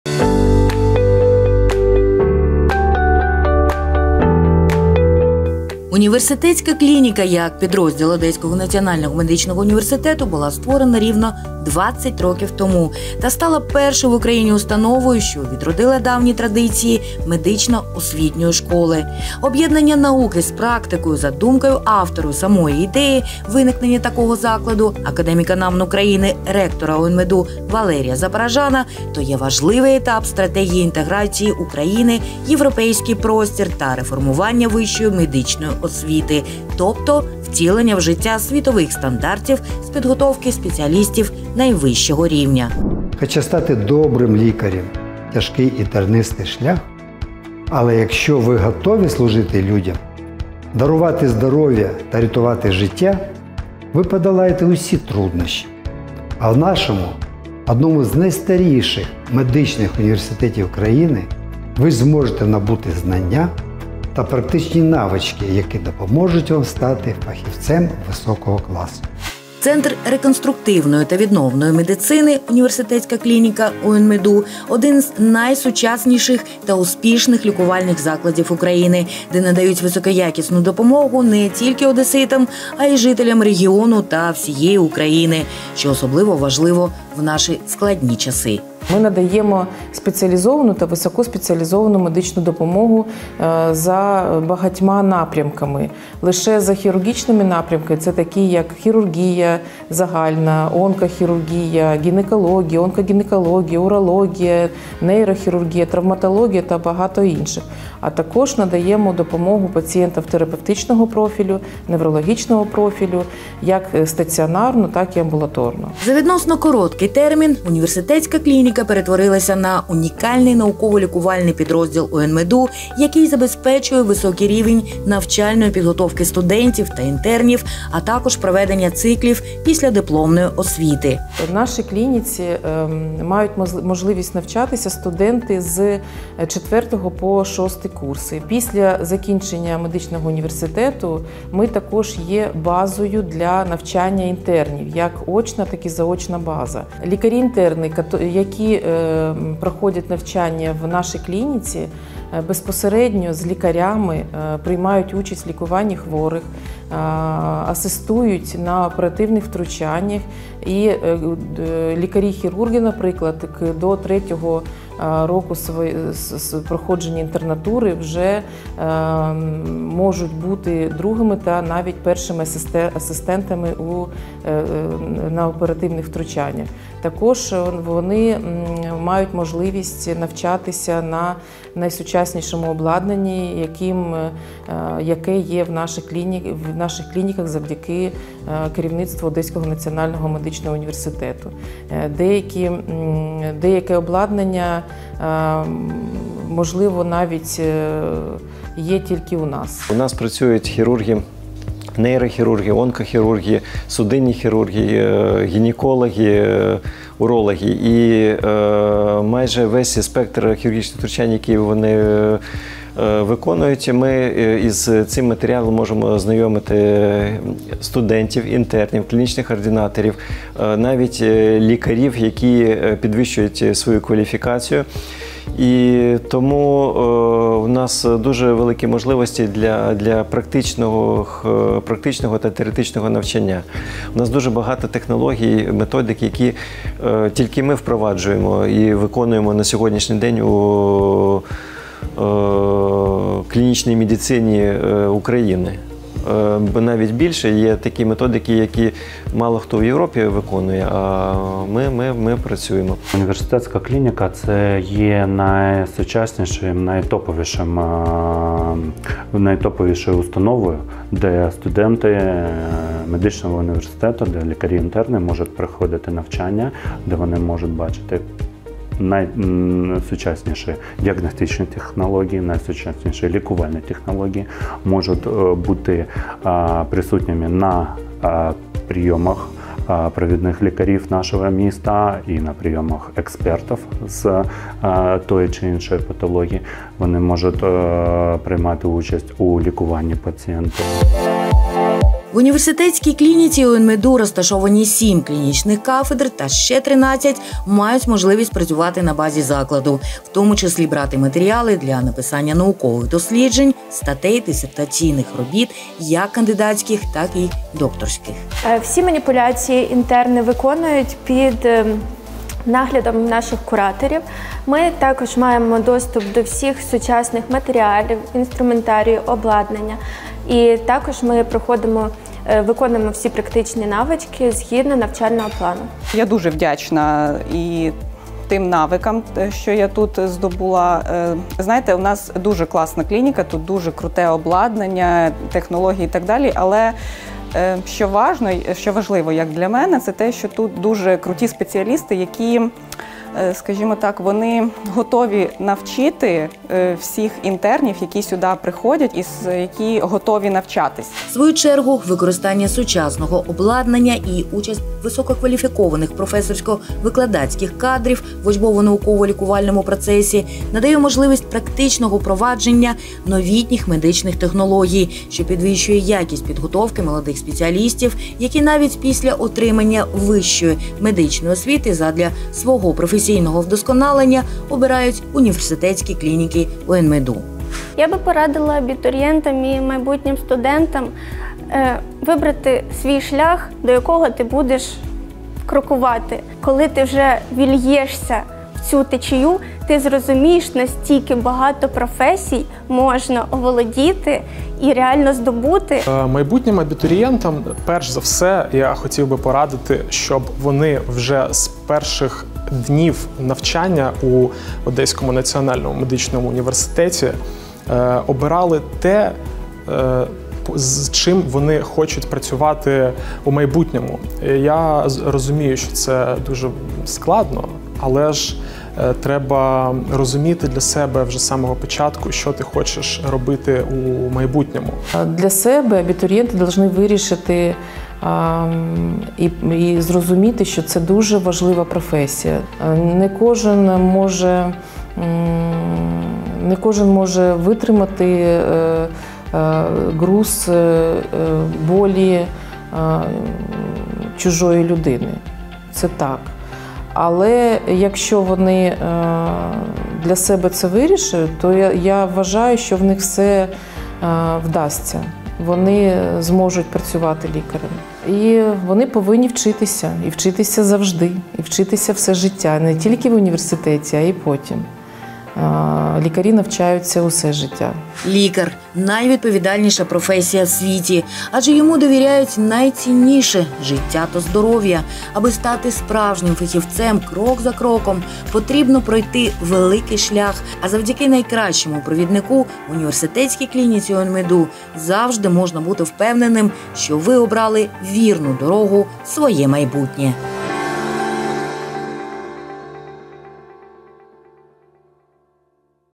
Університетська клініка ОНМедУ: шлях до майбутнього
Університетська клініка, як підрозділ Одеського національного медичного університету, була створена рівно 20 років тому  та стала першою в Україні установою, що відродила давні традиції медично-освітньої школи. Об’єднання науки і практики, за думкою автора самої ідеї виникнення такого закладу академіка НАМН України, ректора ОНМедУ Валерія Запорожана, є важливим етапом стратегії інтеграції України в Європейський простір та реформування вищої медичної освіти, тобто втілення в життя світових стандартів з підготовки спеціалістів найвищого рівня!